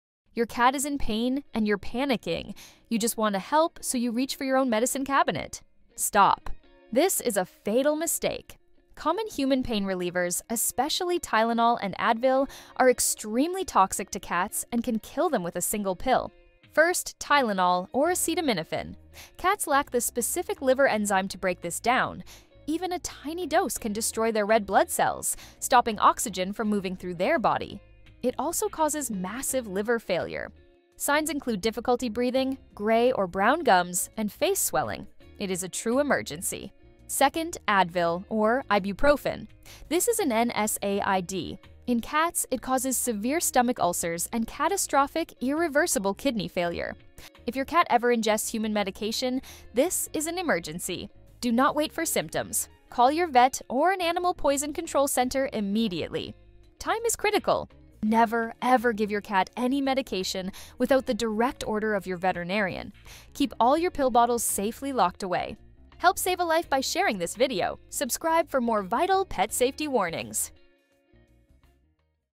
Tylenol & Advil are DEADLY for Cats (A Vet's Warning)
If your cat is in pain, you might be tempted to reach into your own medicine cabinet for a "small dose" of Tylenol or Advil. DO NOT DO THIS. This well-intentioned act can be a fatal mistake.

This vital safety guide explains why common human pain relievers are extremely toxic and deadly to cats. Learn how Acetaminophen (Tylenol) causes liver failure and stops their blood from carrying oxygen, and how NSAIDs (Advil, Aleve) cause kidney failure and stomach ulcers. A single pill can kill.